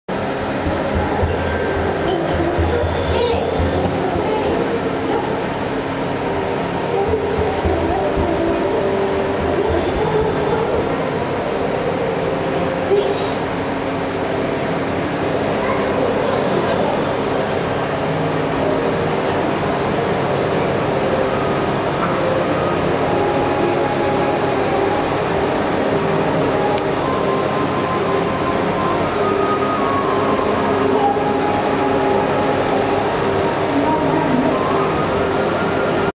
Dugong at Sydney Aquarium
A dugong lounging around at the Sydney Aquarium